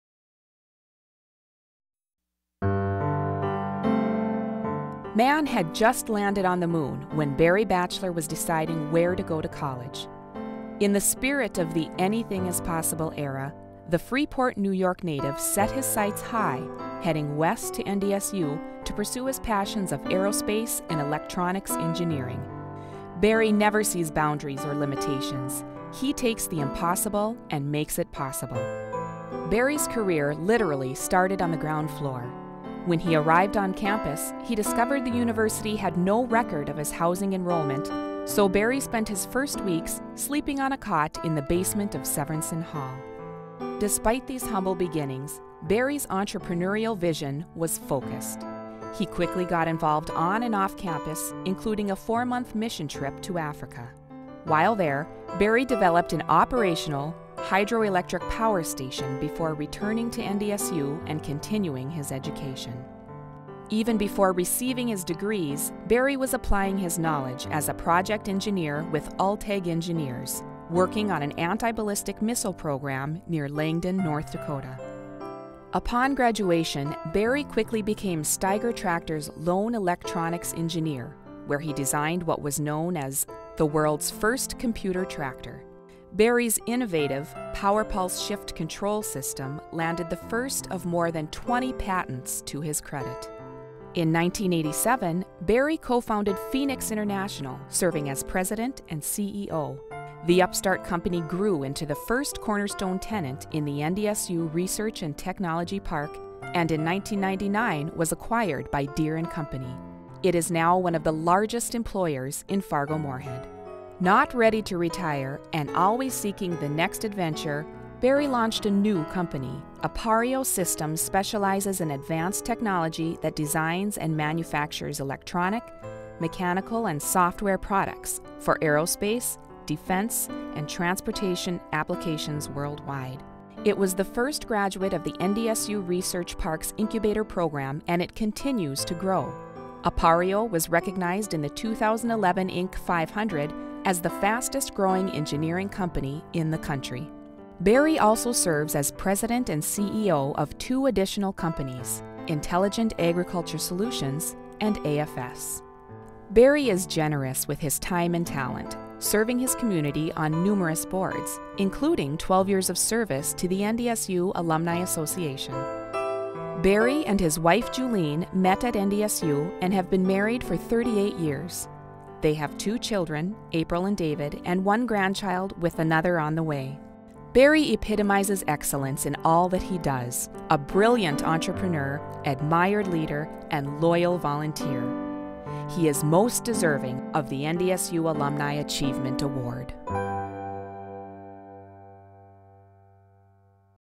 Alumni Achievement Award 2012, Barry D. Batcheller
Barry Batcheller is a 2012 recipient of the NDSU Alumni Achievement Award. Barry has been involved in the founding of four start up companies and is the holder of over 20 patents. He currently serves at the CEO of Appareo Systems, LLC.This award was presented to Barry Batcheller at the Alumni Awards Celebration, April 26, 2012.